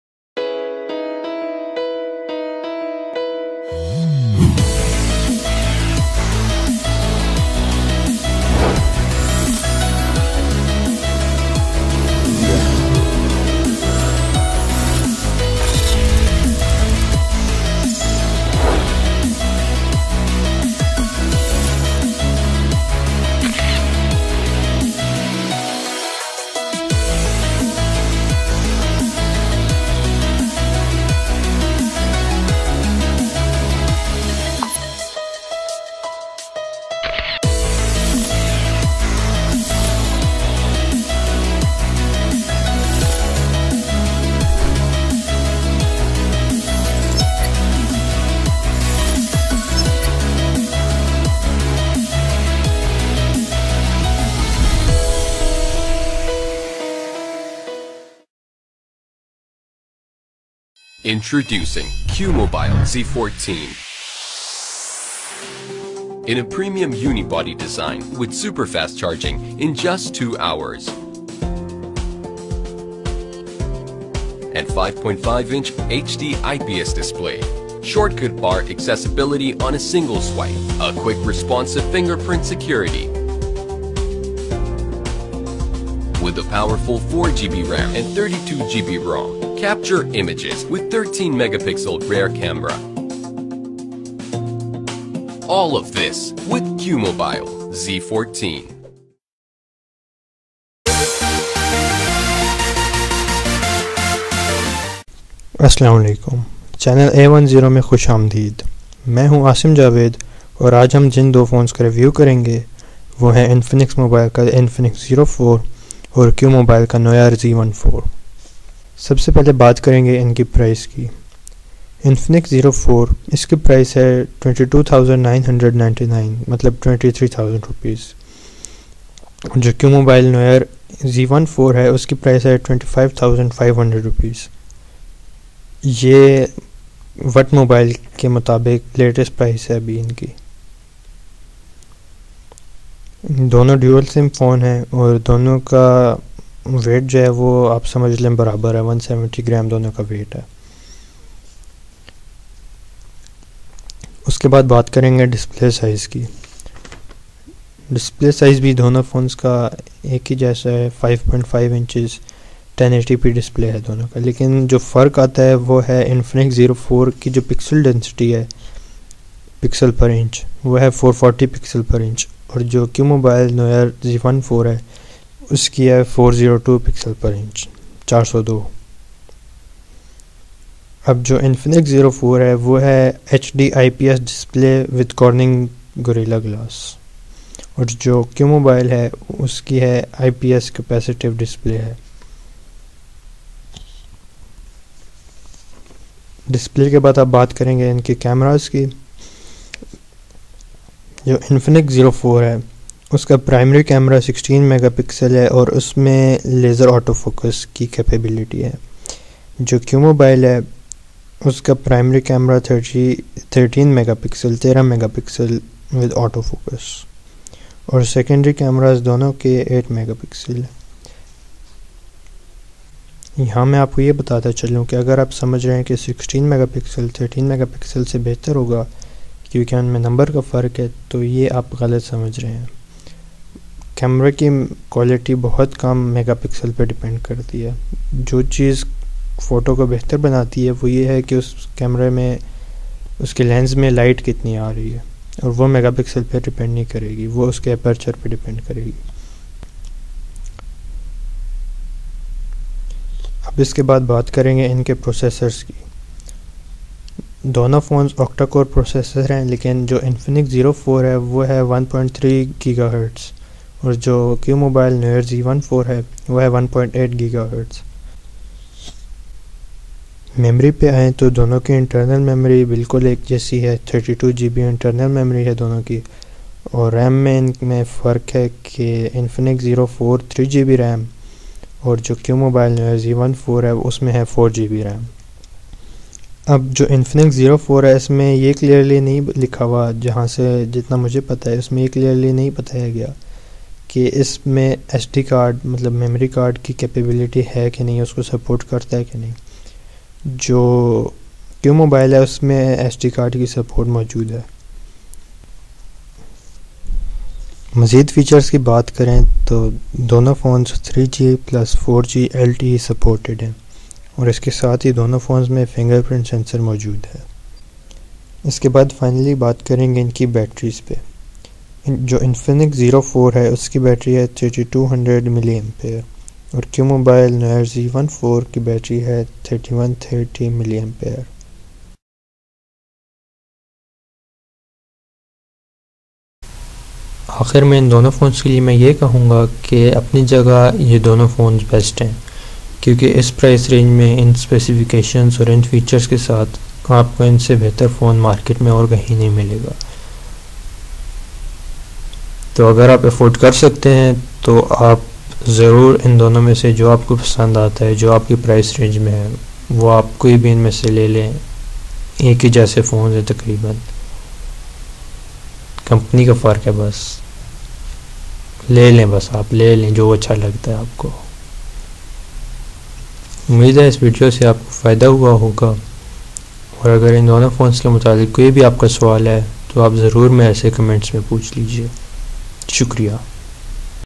Infinix Zero 4 VS QMobile Noir Z14 COMPARISON in URDU
Hey, this is a brief comparison of two big flagship phones from Infinix and QMobile, the Infinix Zero 4 and the Noir Z14.
Both of them are top class phones in very affordable price, with great value for money.

Note: The Infinix Zero 4 has 3GB RAM according to their own website and WhatMobile, whereas this website says 4GB.